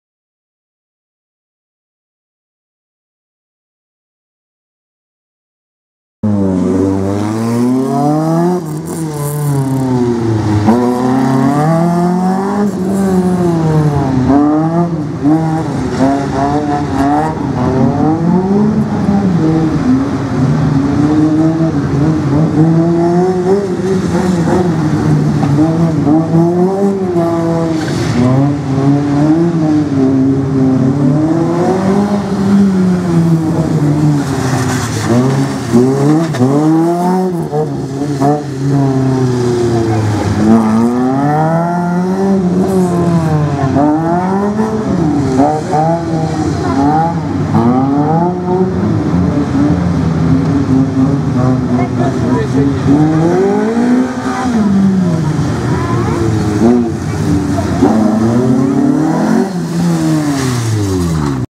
Autowir Rajdowy Ustroń 03.09.2017 Gluza Leszek/Gluza Seweryn
Rajdowy Ustroń / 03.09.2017 przejazd załogi  Gluza Leszek/Gluza Seweryn Honda Civic 1.3  podczas AUTOWIR